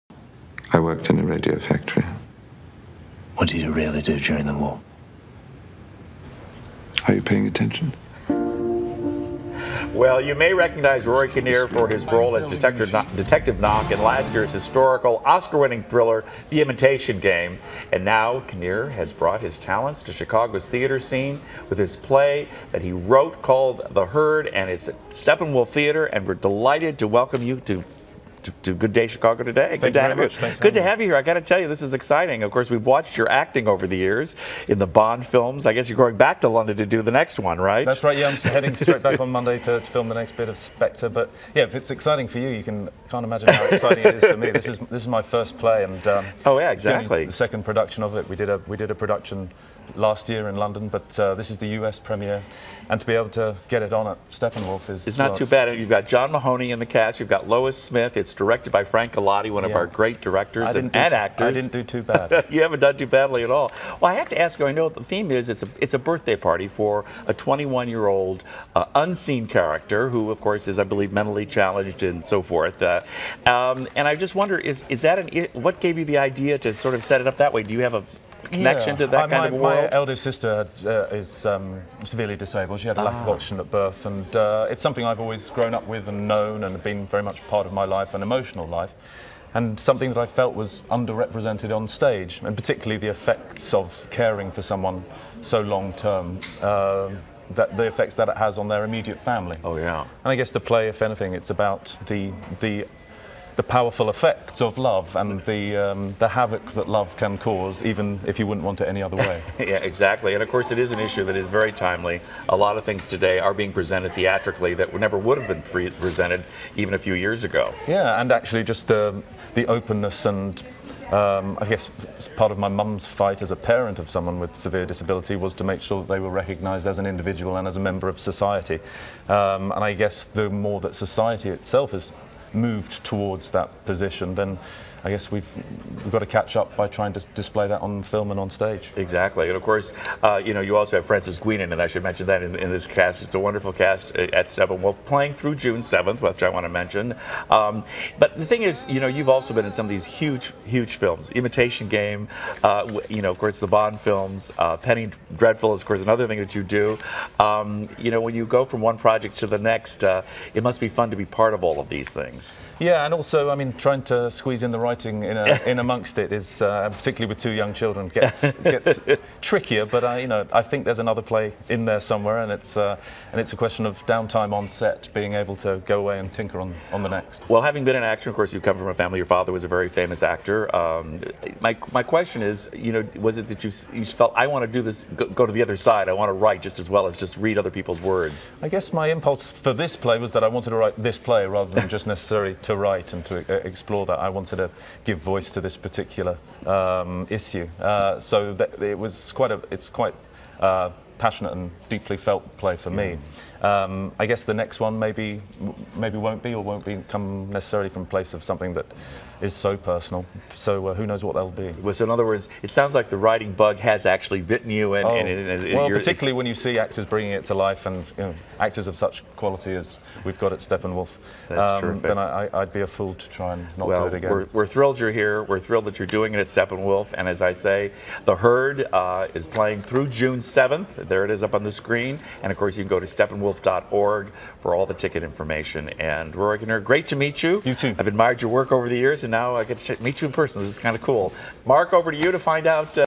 'The Herd' playing at Steppenwolf Theatre
Actor Rory Kinnear is starring in a new play at Steppenwolf called "The Herd."